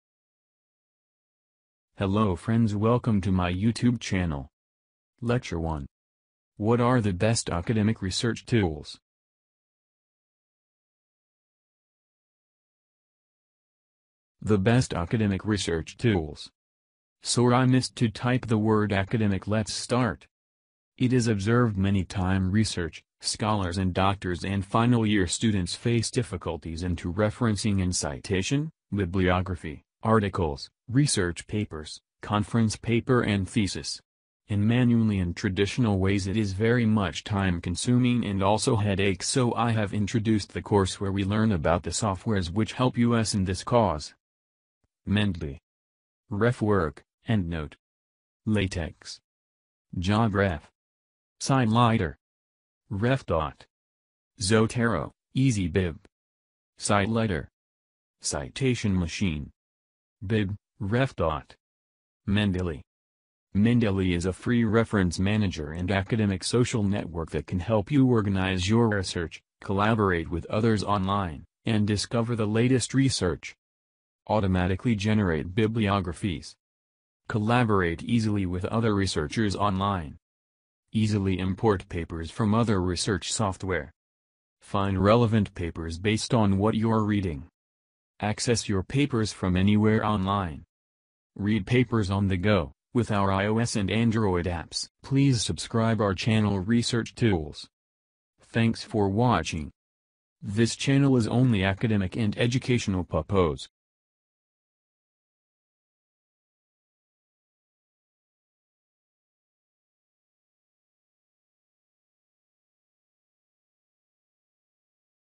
Best academic research tools to write a great research papers For Mphil/PhD/Scholars/OTHERS
BEST RESEARCH TOOLS
THE BEST ACADEMIC  RESEARCH TOOLS
MOSTLY FREE Citations, Bibliographies and Cross References
IT IS OBSERVED MANY TIME RESEARCH, SCHOLARS AND DOCTORS AND FINAL YEAR STUDENTS FACE DIFFICULTIES IN TO REFERENCING AND CITATION,BIBLIOGRAPHY (ARTICLES, RESEARCH PAPERS,CONFERENCE PAPER AND THESIS).  IN MANUALLY AND TRADITIONAL WAYS  IT IS VERY MUCH TIME CONSUMING AND ALSO HEADACHE SO I HAVE INTRODUCED THE COURSE WHERE WE LEARN ABOUT THE  SOFTWARE'S WHICH HELP US IN THIS CAUSE.
INTRODUCTION(ONLINE REFERENCING AND CITATION SOFTWARES)LECTURE ONE
HOW TO CREATE ACCOUNT IN THE MENDELY LECTURE TWO
INSTALLATION OF MENDELY AND ITS OVERVIEW LECTURE THREE
INSTALLATION OF WEB CRAWLER AND MICROSOFT PLUGIN FOR MENDELY LECTURE FOUR
HOW TO CITE AND BIBLOGRAPHY IN MENDELY USING DIFFRENT REFRENCE STYLES(IEEE,APA)LECTURE FIVE